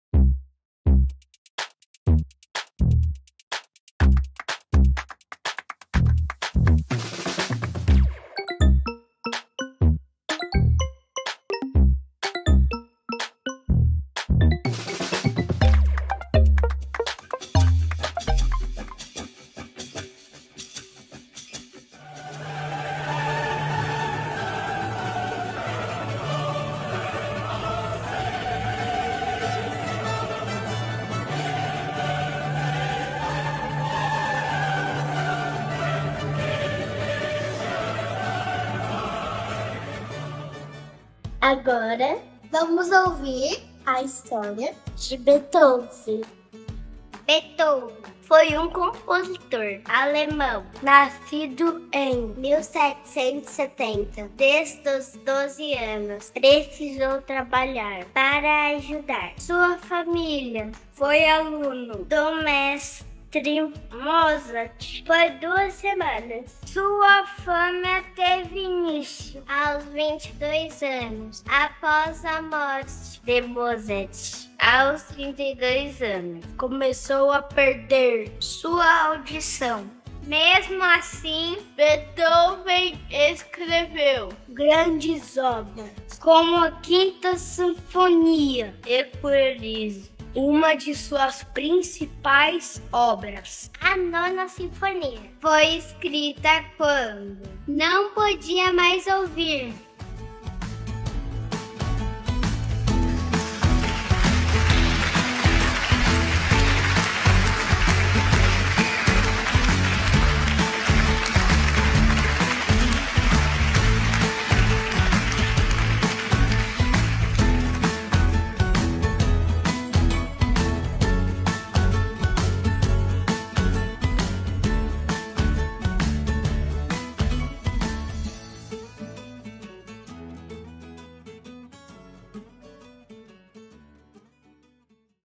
3ºMB (CAVM/2024) FRICCIONADAS
Vídeo exibido no Recital CeMASP do dia 23/10/24, com imagens dos alunos do 3ºMB do Colégio Adventista Vila Matilde, no auditório do CAVM (SP), no módulo “Cordas Friccionadas”.

Esta apresentação faz parte da série “DINÂMICA MUSICAL” usada na matriz curricular de Arte/Música do ensino fundamental do Colégio. Cada ano, uma família musical diferente é estudada e junto dela o ensino técnico/prático de um instrumento que a represente, além do conhecimento visual/auditivo de outros oito instrumentos da mesma família, quatro compositores e um instrumento criativo.